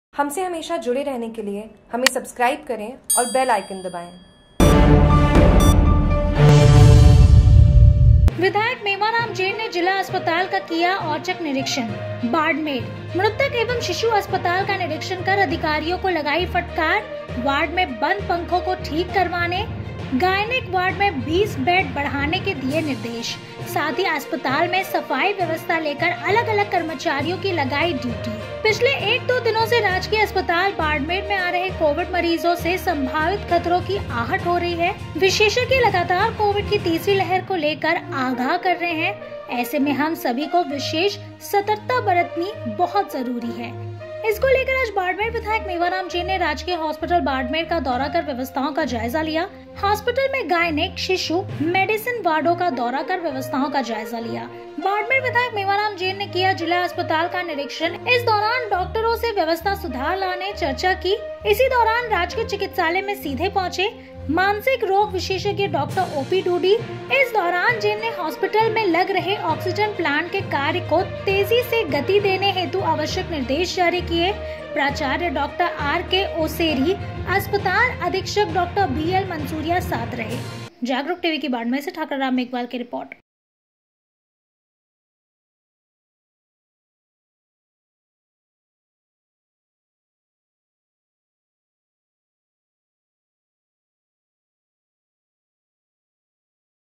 बाड़मेर : विधायक मेवाराम जैन ने जिला अस्पताल का किया औचक निरीक्षण
बाडमेर विधायक मेवाराम जैन ने किया जिला अस्पताल का निरीक्षण दौरान डाक्टरो से व्यवस्था सुघार लाने चर्चा कीइसी दौरान राजकीय चिकित्साल मे पहुचे
सीघे पहुचे
-मानसिक रोग विशेषज्ञ डॉक्टर ओपी डूडी ,,         इस दौरान जैन ने हॉस्पिटल में लग रहे ऑक्सीजन प्लांटों के कार्य को तेजी से गति देने हेतु आवश्यक निर्देश जारी किये प्राचार्य ड़ॉ आरके आसेरी,अस्पताल अधीक्षक ड़ॉ बीएल मंसुरिया   साथ रहे।